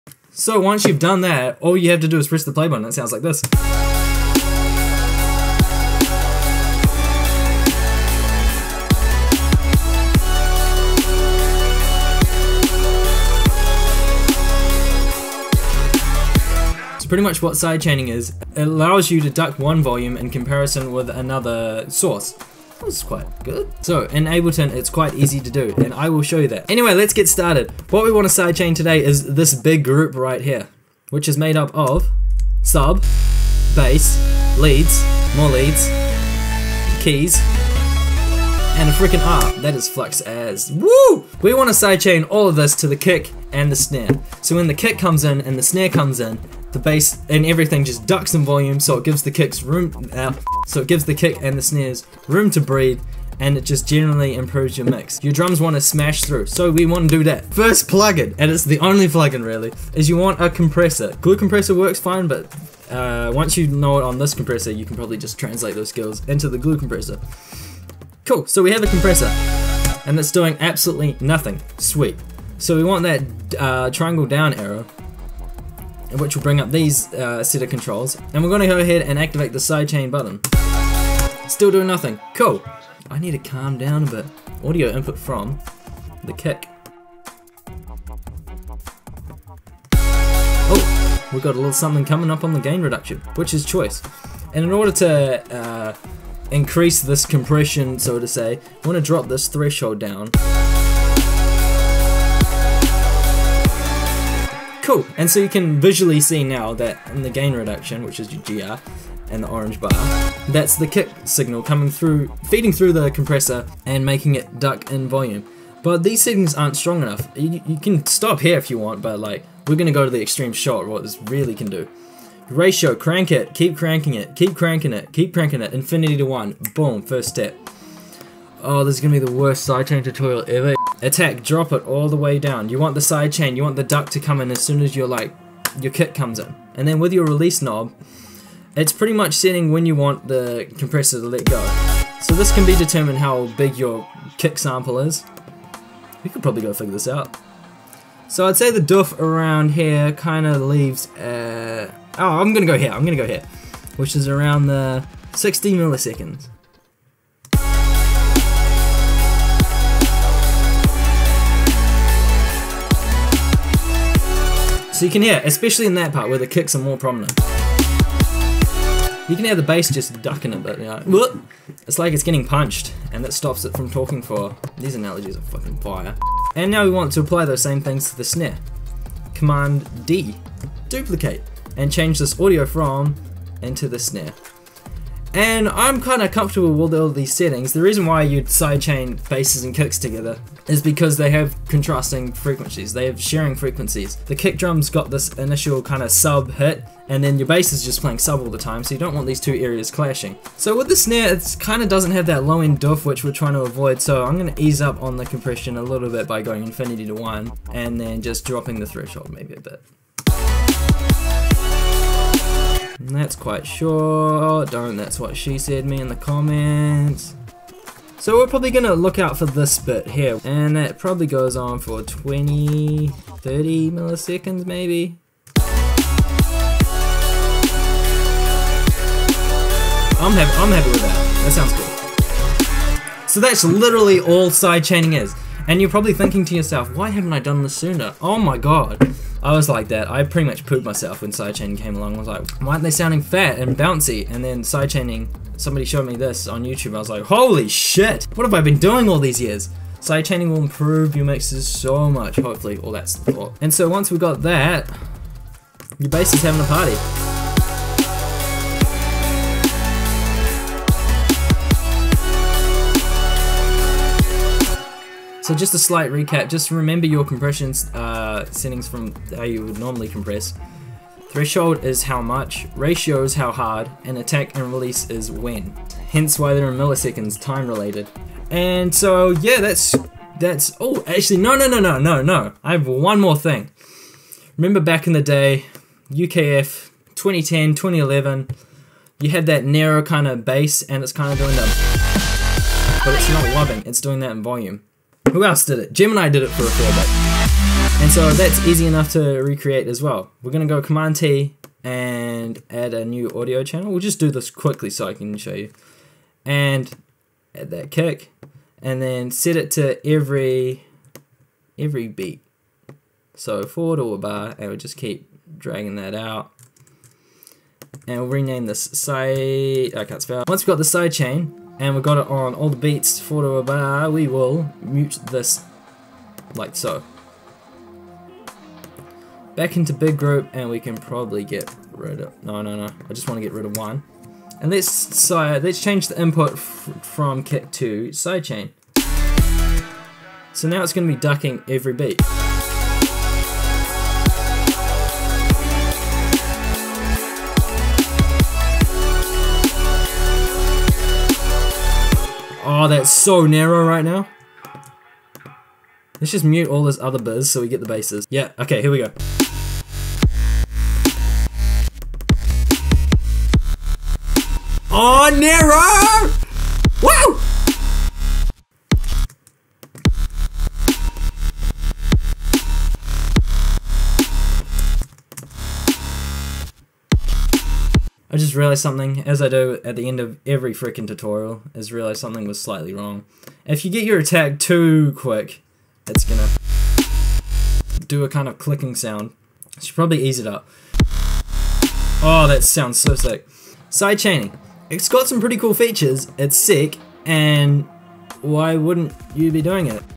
Ableton Tutorial: How To Sidechain Bass to Drums in Dubstep & More
Ableton Tutorial on how to use sidechain compression inside a dubstep tune.
Been wanting to do this for a while now. And well, we got there. So, in this tutorial I cover sidechain compression to clean up the bass frequencies and add punch and groove to a melodic dubstep tune inspired by flux pavilion. Also, towards the end, I dive into a sidechain effect to get your basses sounding like 2010/2011 Nero, Gemini etc.

So if you dont know what sidechaining is, this video is a must!!